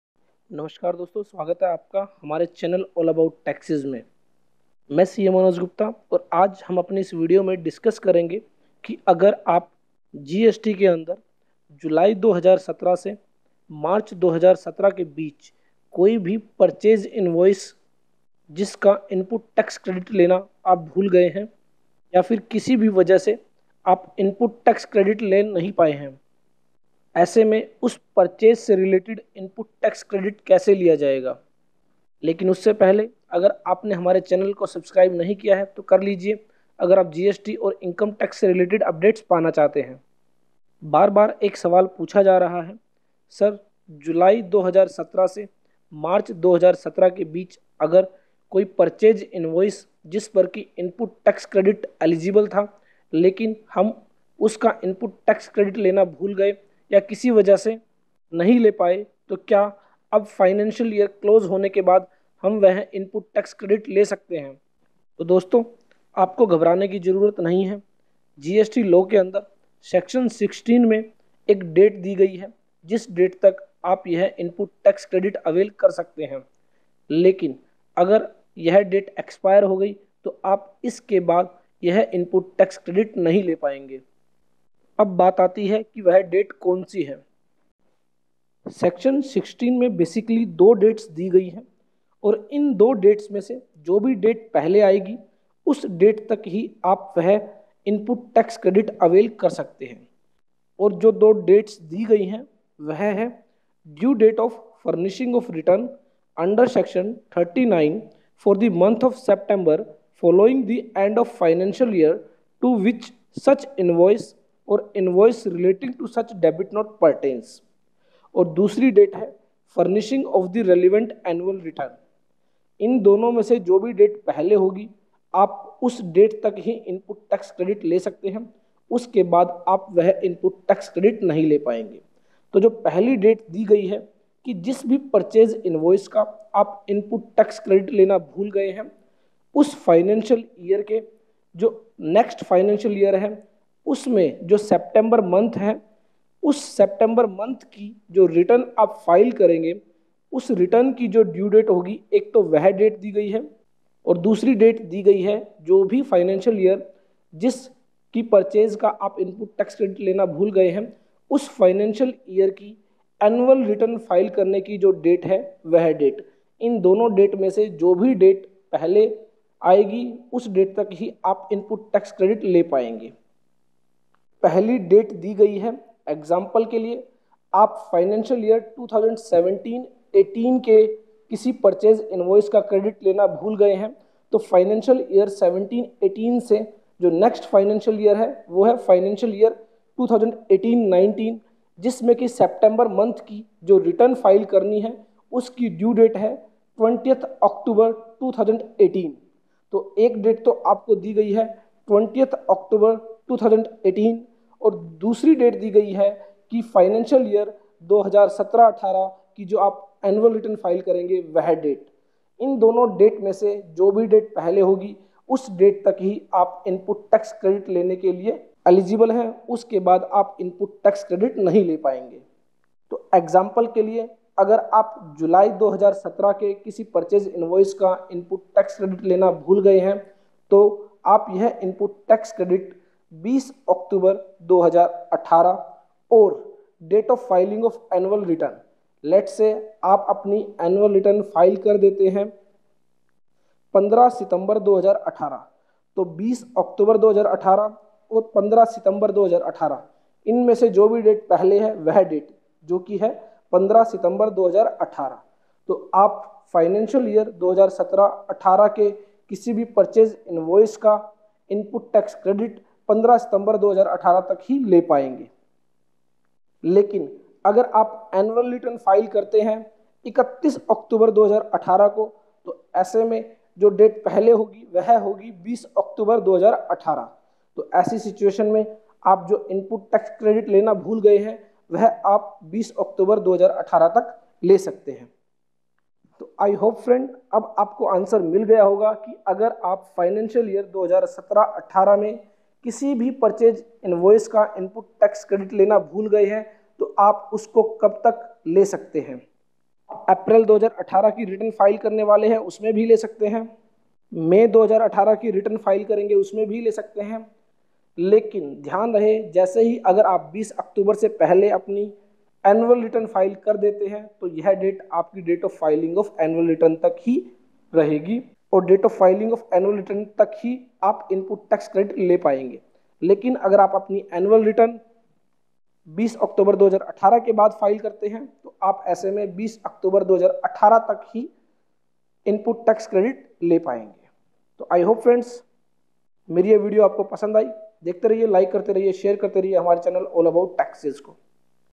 अगर पिछले साल भूल गए है INPUT TAX CREDIT लेना तो इस साल क्लेम करे !!! CA MANOJ GUPTA !!!
(4) A registered person shall not be entitled to take input tax credit in respect of any invoice or debit note for supply of goods or services or both after the due date of furnishing of the return under section 39 for the month of September following the end of financial year to which such invoice or invoice relating to such debit note pertains or furnishing of the relevant annual return, whichever is earlier.